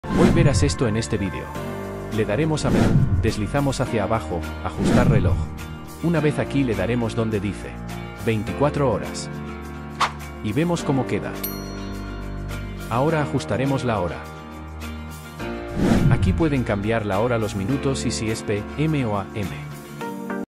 Cómo ajustar la HORA Poner forma de 24 Horas o quitarlo CARRO ford Focus 2014
Aprenderás a ajustar la hora y a poner o quitar las 24 horas de tu carro ford focus 2014

Hecho con Capcut

Musica utilizada